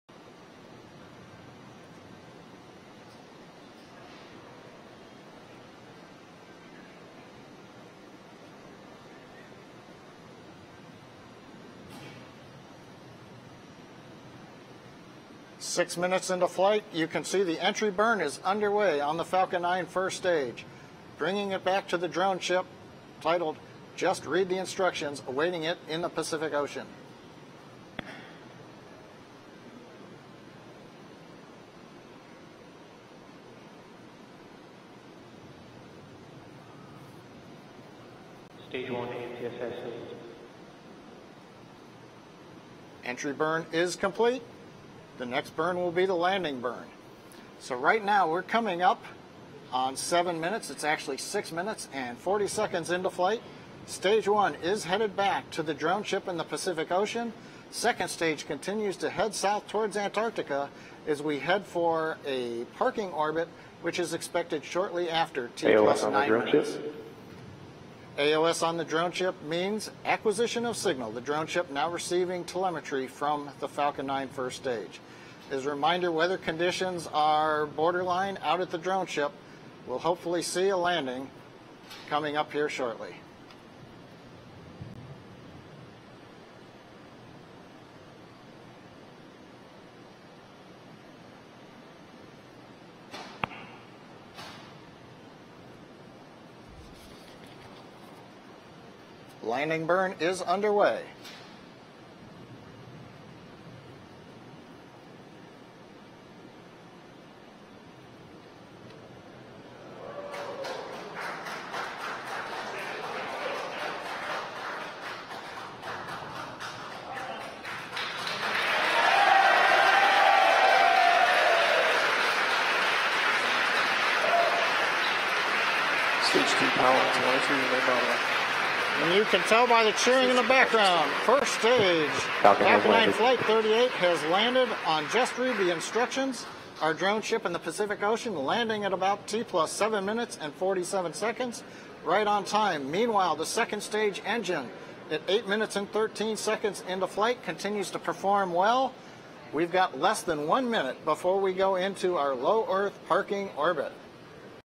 SpaceX Lands on Drone Ship After Foggy Launch
The Falcon 9 first stage landed on a drone ship in the Pacific Ocean after launching 10 Iridium-2 satellites.

Credit: SpaceX